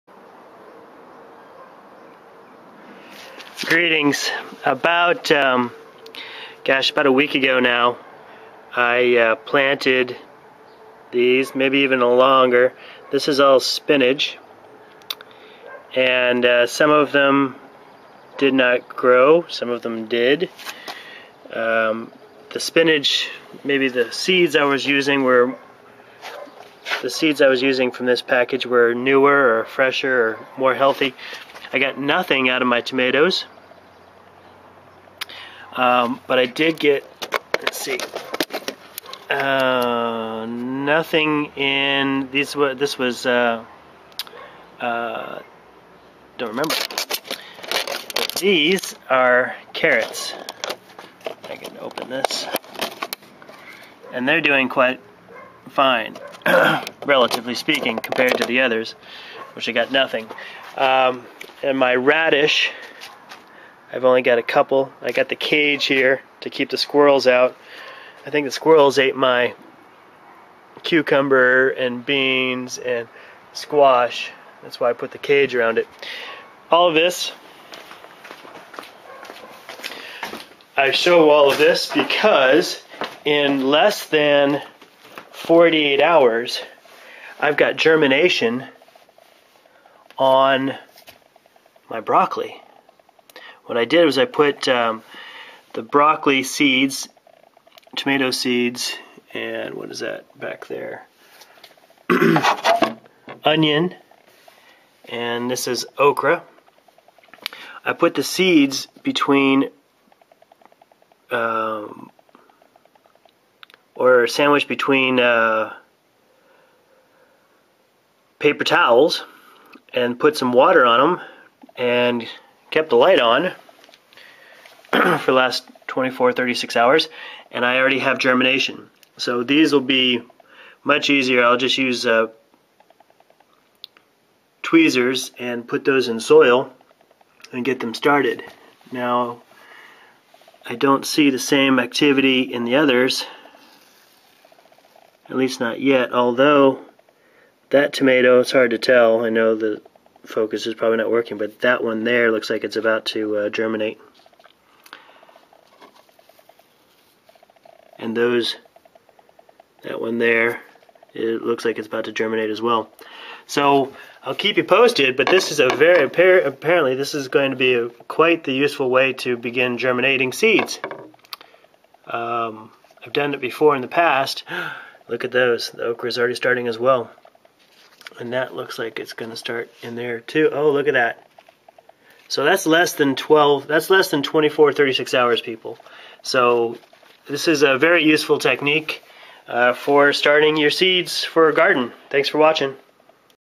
Gardening update: germination in under 48 hours
Gardening update. Using simple paper towels on a plate with a 100 watt light bulb I was able to induce germination in two species in under 48 hours.